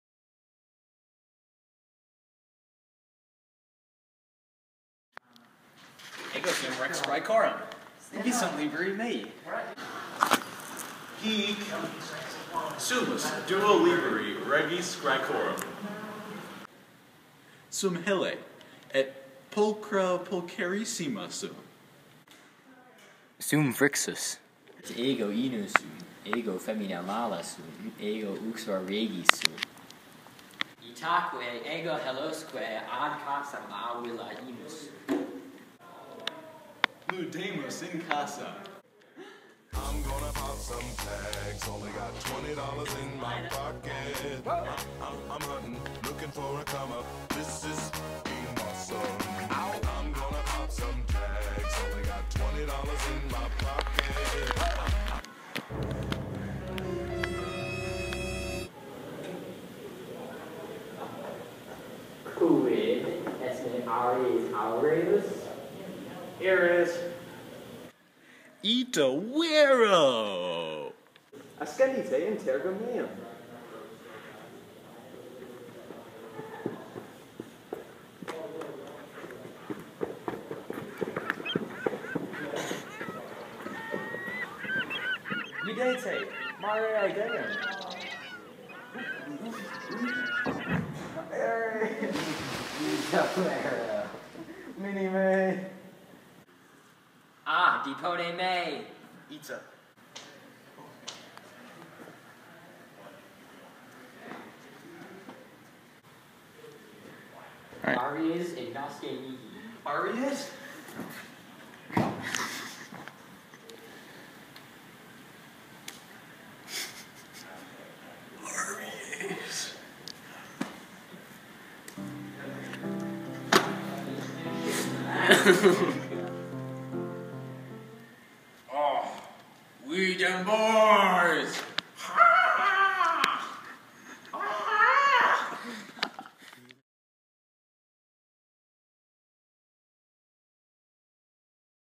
Lingua Latina, Phrixus et Helle
An all-Latin version of the myth of the Fabulae Syrae version of the myth of Phrixus et Helle by BHS Latin III Students.